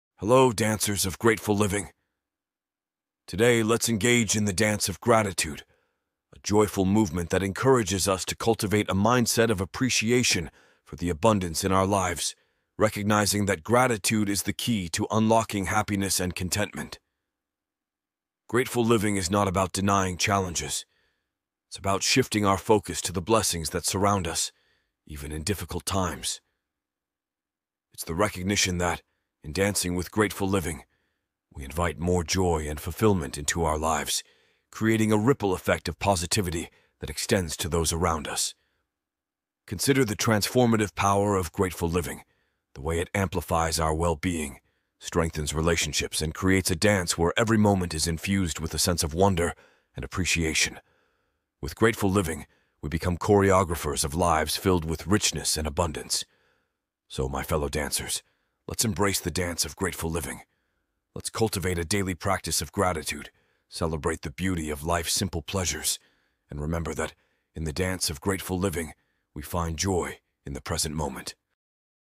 The Dance of Grateful Living
Hello, dancers of grateful living! Today, let's engage in the dance of gratitude—a joyful movement that encourages us to cultivate a mindset of appreciation for the abundance in our lives, recognizing that gratitude is the key to unlocking happiness and contentment. Grateful living is not about denying challenges; it's about shifting our focus to the blessings that surround us, even in difficult times. It's the recognition that, in dancing with grateful living, we invite more joy and fulfillment into our lives, creating a ripple effect of positivity that extends to those around us. Consider the transformative power of grateful living—the way it amplifies our well-being, strengthens relationships, and creates a dance where every moment is infused with a sense of wonder and appreciation. With grateful living, we become choreographers of lives filled with richness and abundance. So, my fellow dancers, let's embrace the dance of grateful living. Let's cultivate a daily practice of gratitude, celebrate the beauty of life's simple pleasures, and remember that, in the dance of grateful living, we find joy in the present moment.